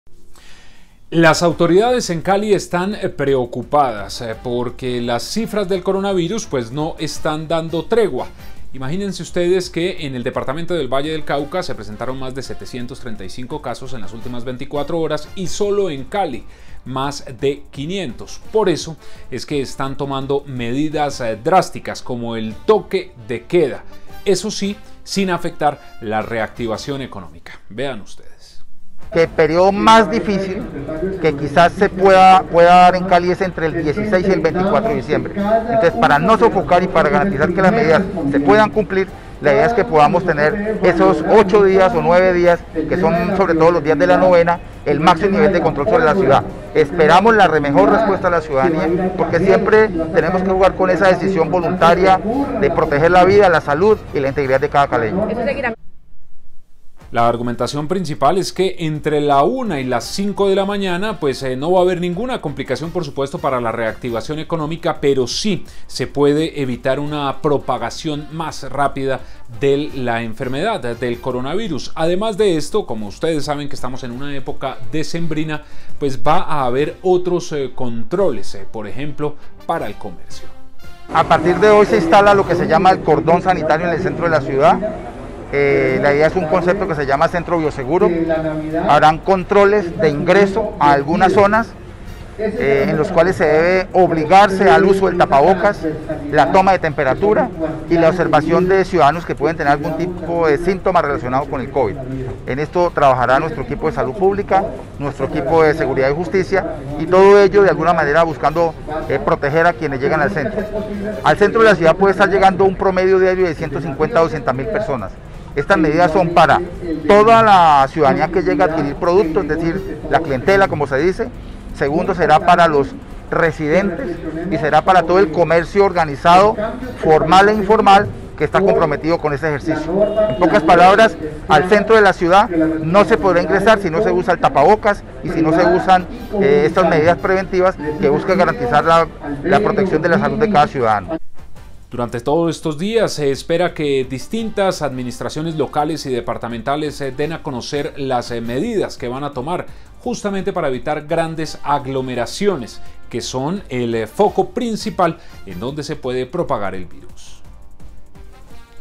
¿Por qué habrá ley seca y toque de queda en Cali? | Semana Noticias
Desde este jueves y hasta el próximo 16 de diciembre habrá ley seca y toque de queda en Cali. Las medidas buscan reducir la circulación de ciudadanos para frenar la expansión del coronavirus.